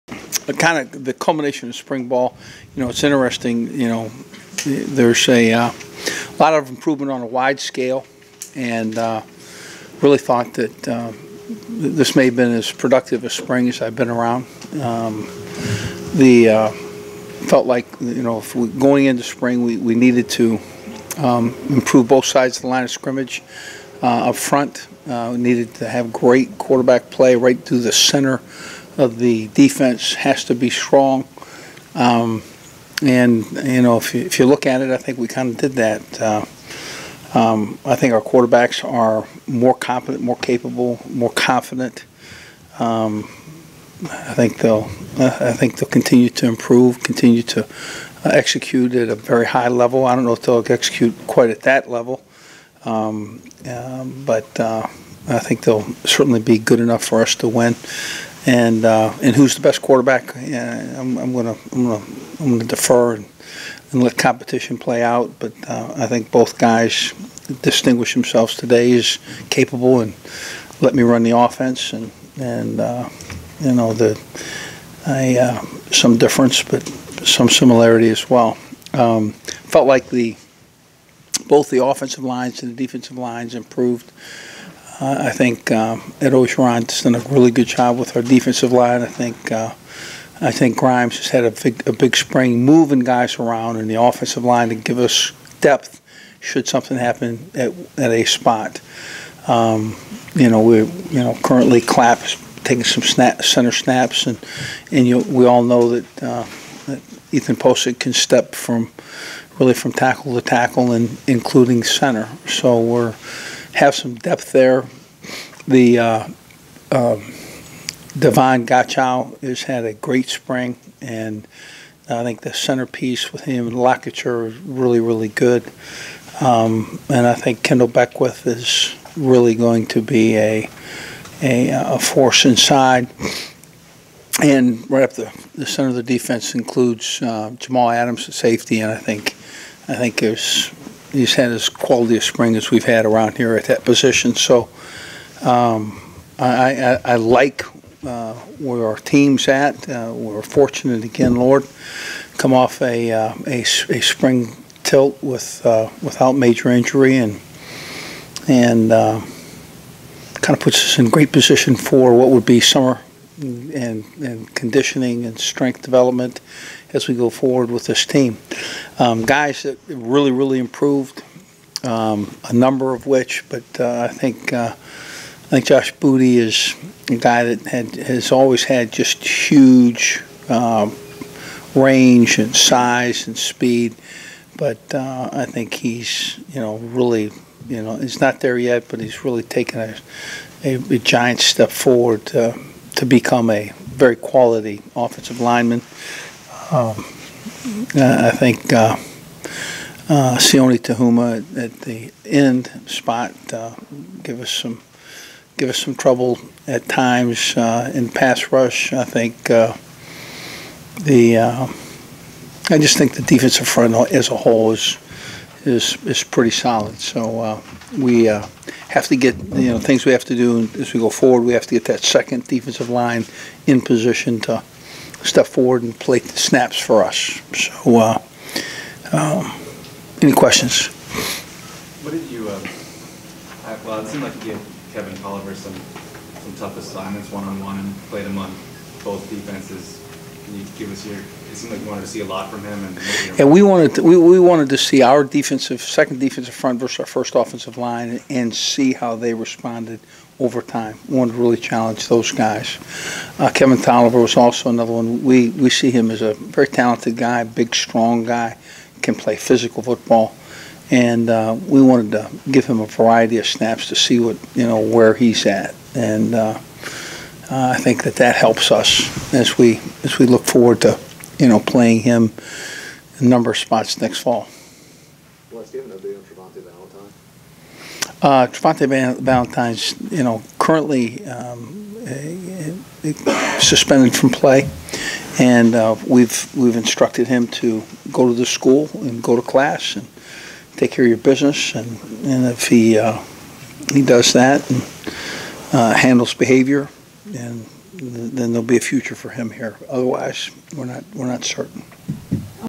LSU coach Les Miles said he will let the QB competition 'play out' | Video
LSU coach Les Miles talks to the media after Saturday&apos;s spring game.